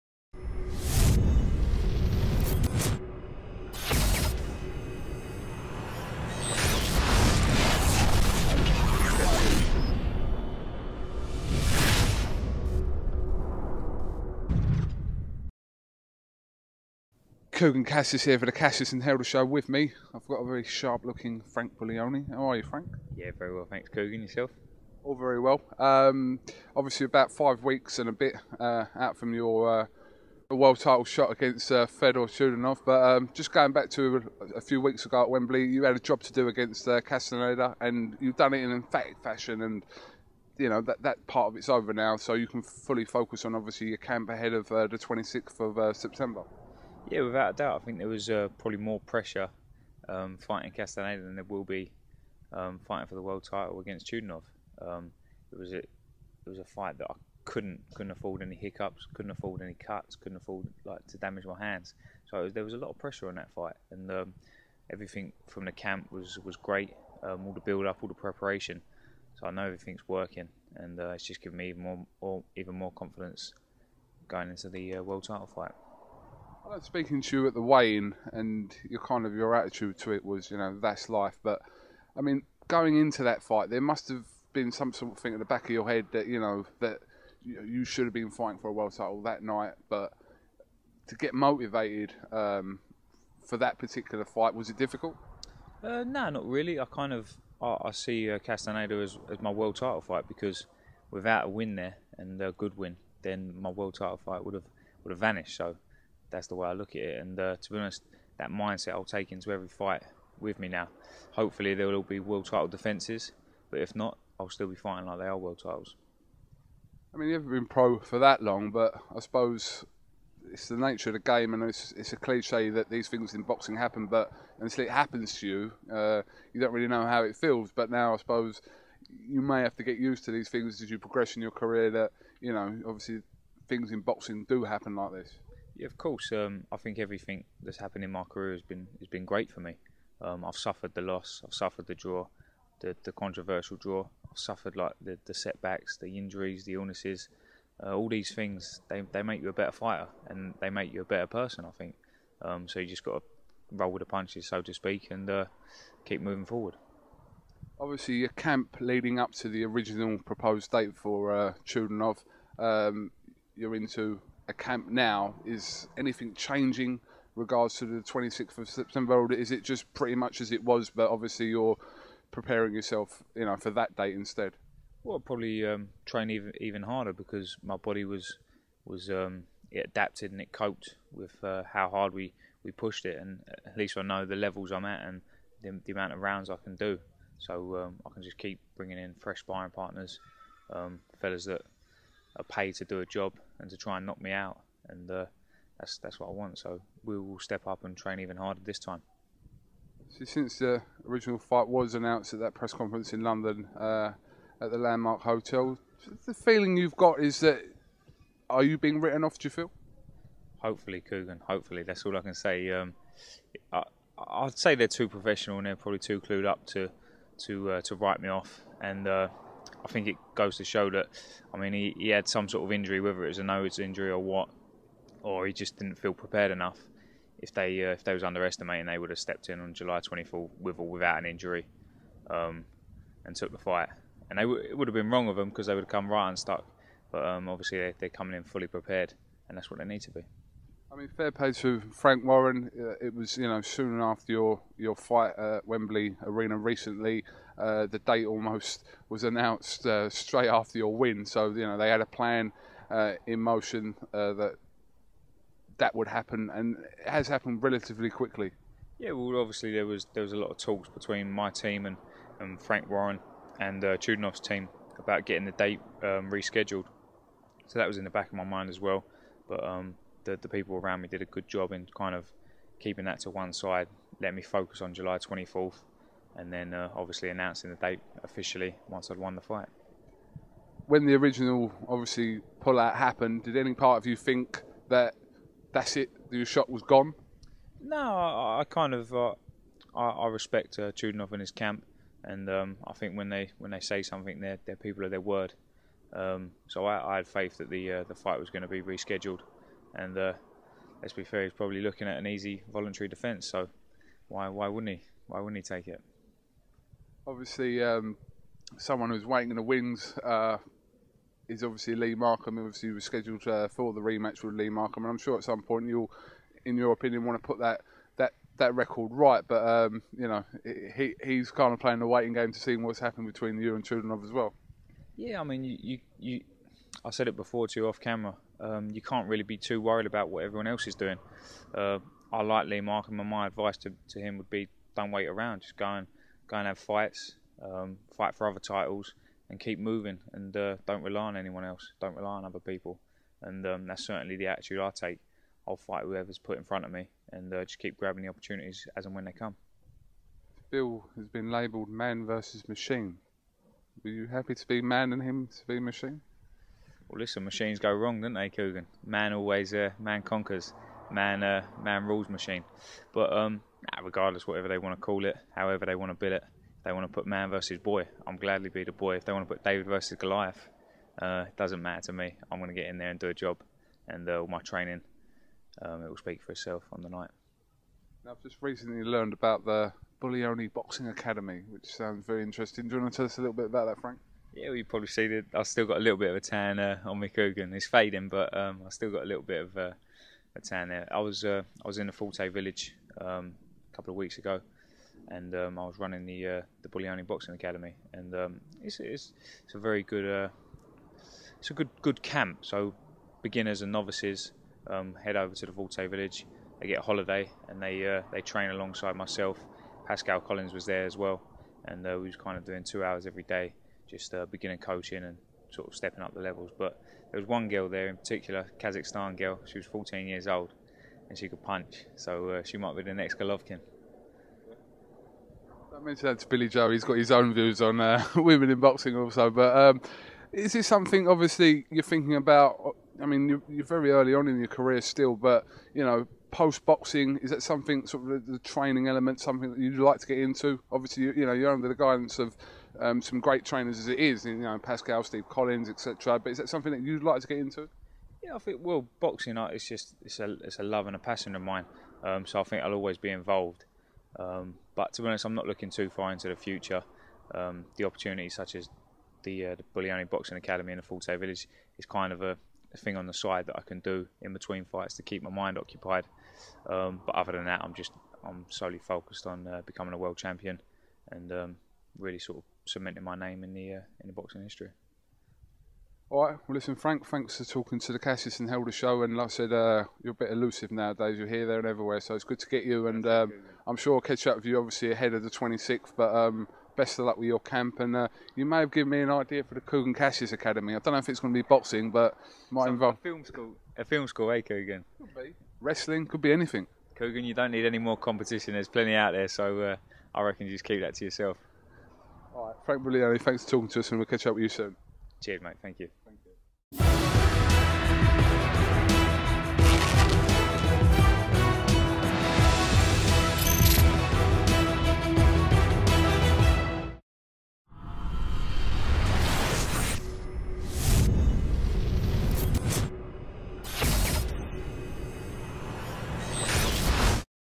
FRANK BUGLIONI - 'HOPEFULLY THEY ARE WRITING ME OFF AGAINST CHUDINOV!' / CHUIDNOV v BUGLIONI
FRANK BUGLIONI - 'HOPEFULLY THEY ARE WRITING ME OFF AGAINST CHUDINOV!' / CHUIDNOV v BUGLIONI - SEPT 26